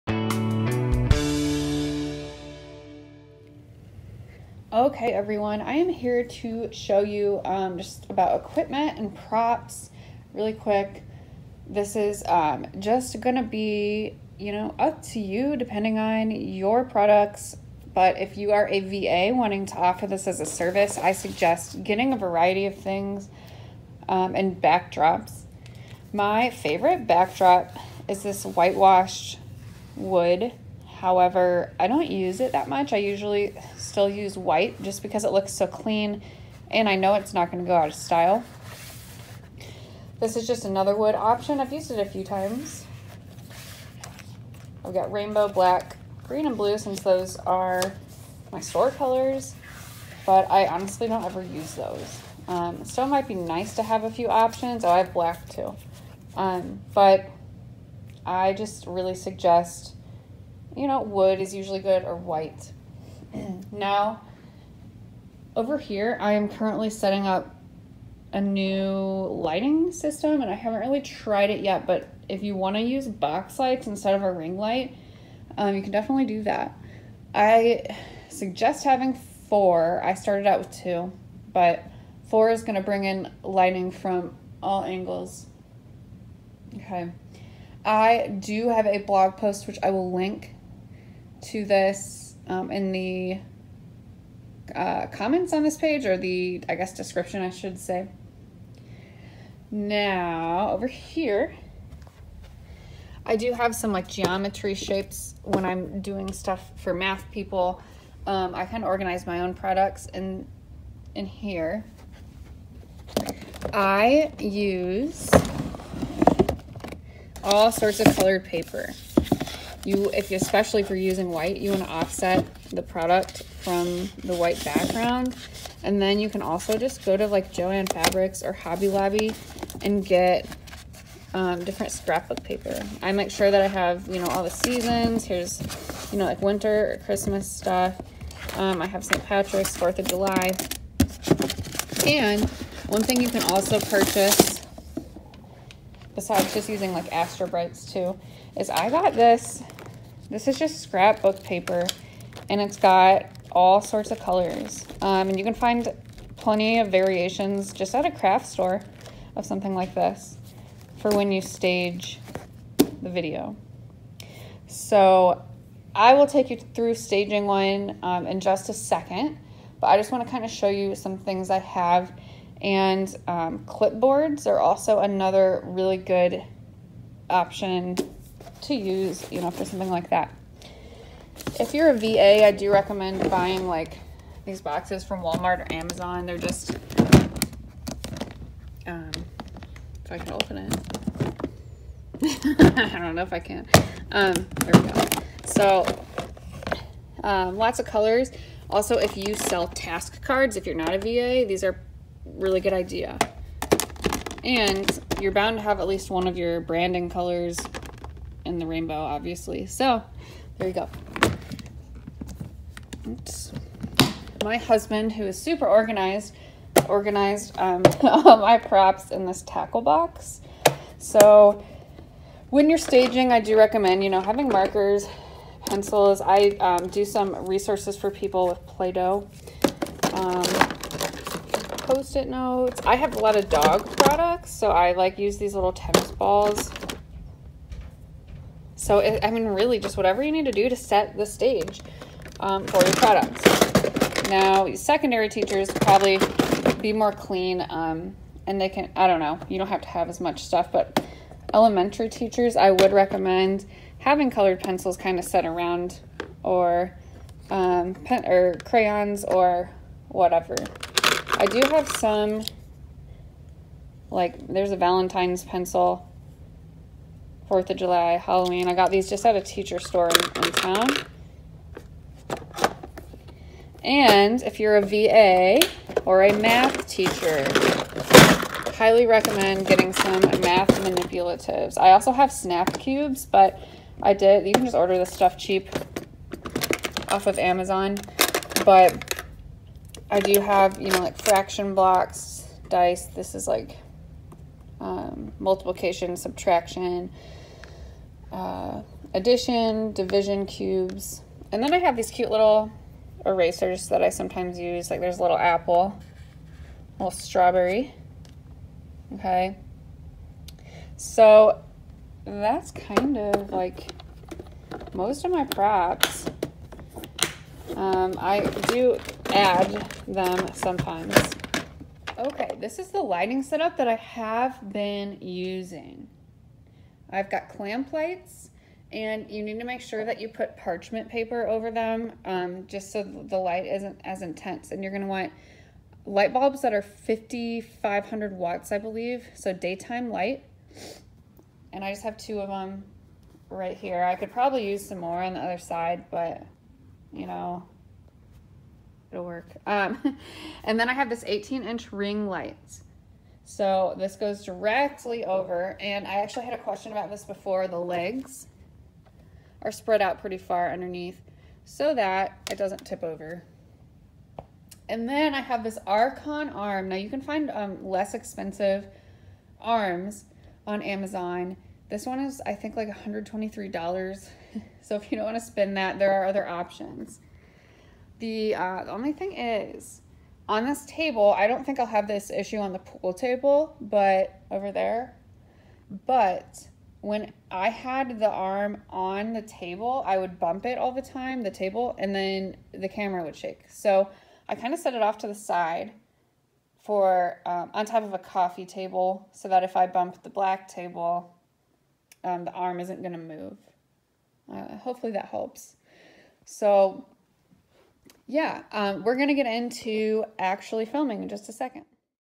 Equipment for Taking Product Videos | Teachers Pay Teachers Preview Videos
Unsure of where to start when it comes to taking pictures of your TPT resources? It can all be overwhelming, but you know you need to get some video previews up and going in your Teachers Pay Teachers store. I am here to help!
My name is Chloe, and I have taken thousands of videos for teacher authors like yourself. This video will show you what I use to film crisp, clean videos for TPT sellers. It's also a free preview of my Premiere Previews program. You can join the waitlist here: